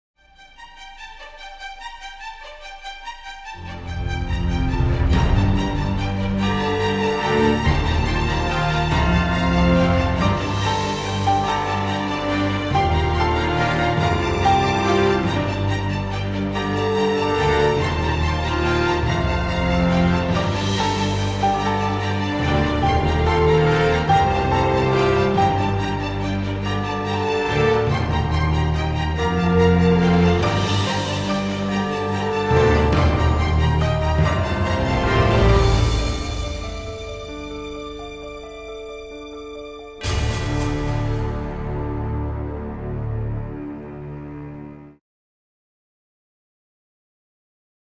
gen wolności gene of freedom ENGLISH SUBTITLES
produced by Trailer and More
Gen Wolności, spot kampani społecznej, Trailer And More, Instytut Lecha Wałęsy, Studio Zakład, Narodowe Centrum Kultury